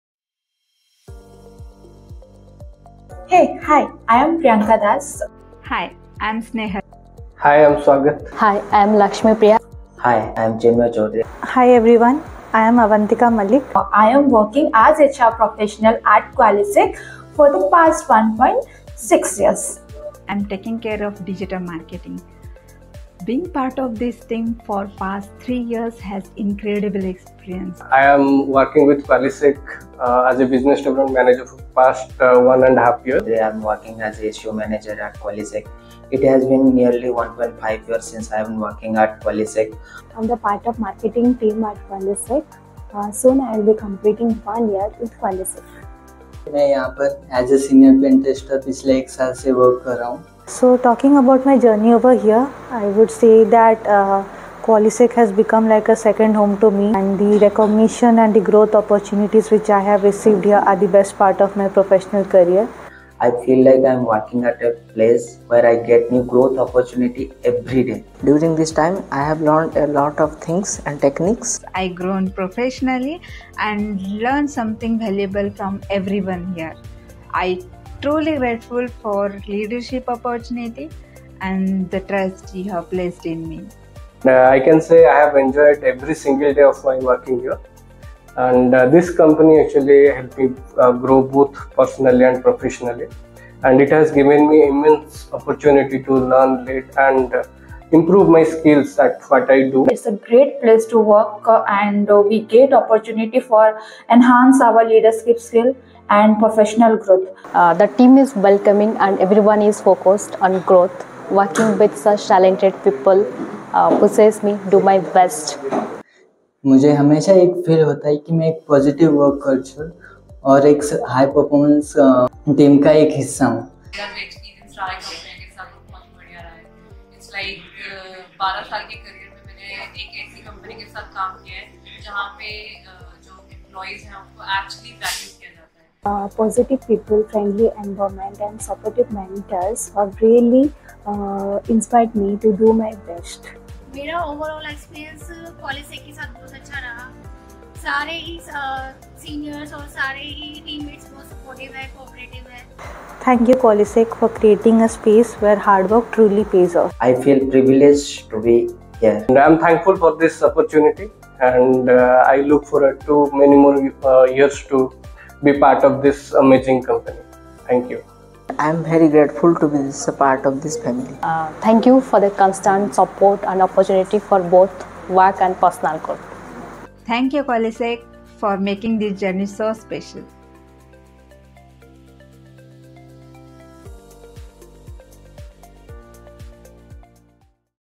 Employee Experience Journey at Qualysec | Employee Testimonial Video
"Hear directly from our team about their journey with Qualysec! In this video, an employee shares personal insights into their day-to-day experience, the supportive work culture, and what makes Qualysec a place to grow and excel."